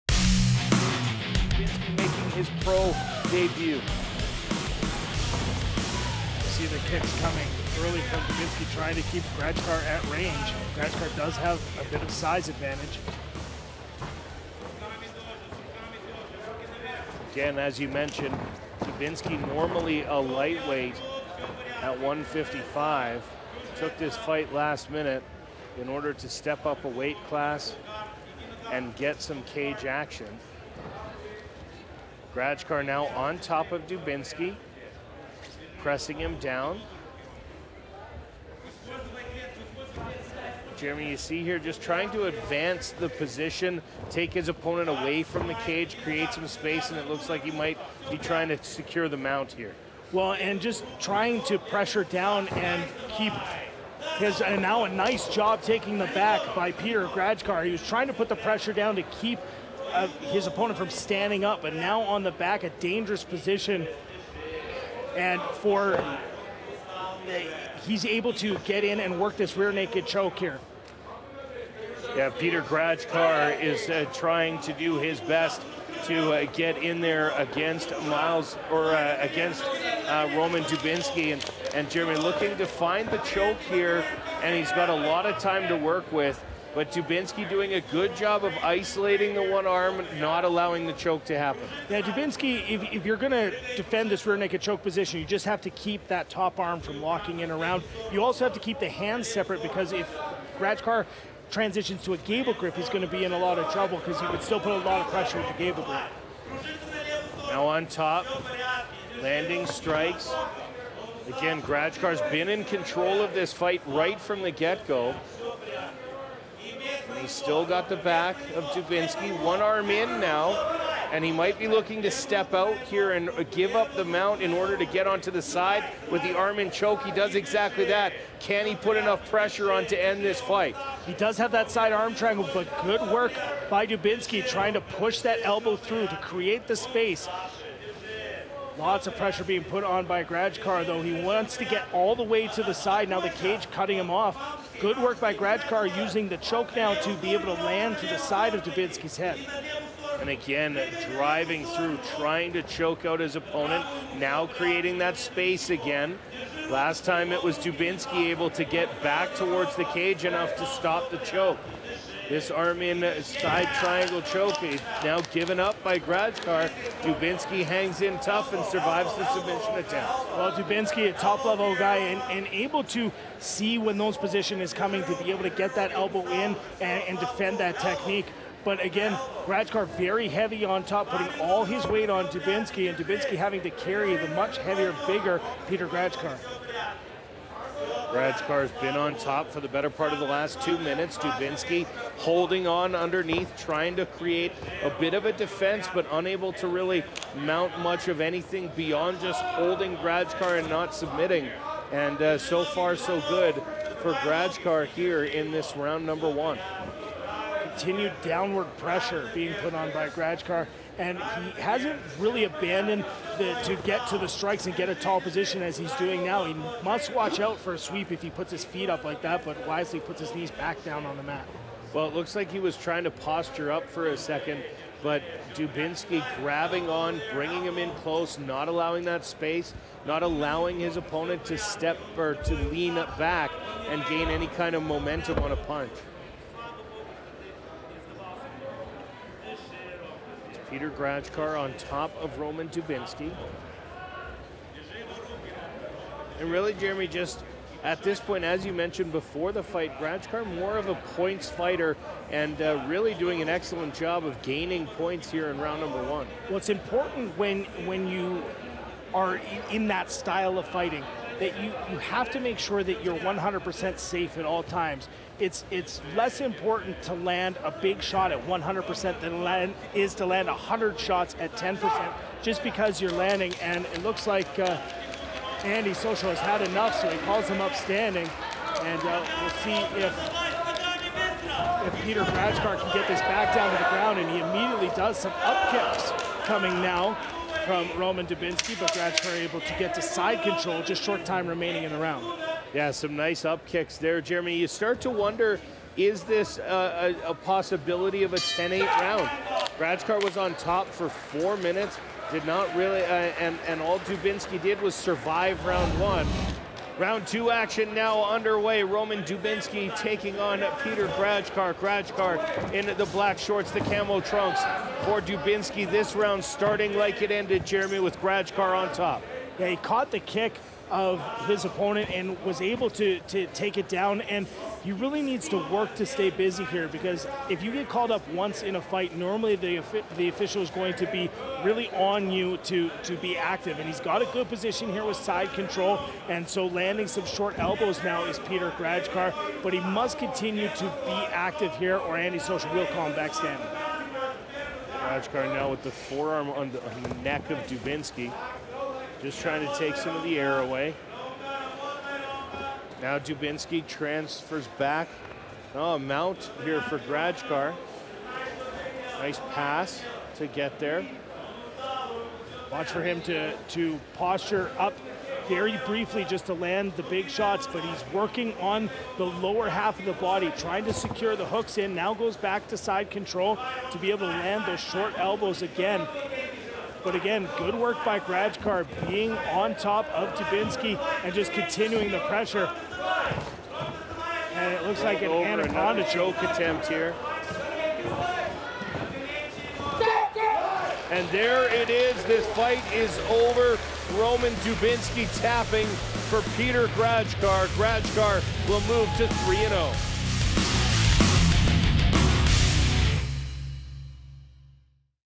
Peter Grajcar vs Roman Dubinski | MMA | Hard Knocks Fighting | HKFC 50
Professional and Amateur fighters from across North America battle it out in Hard Knocks' cage.  Peter Grajcar v. Roman Dubinski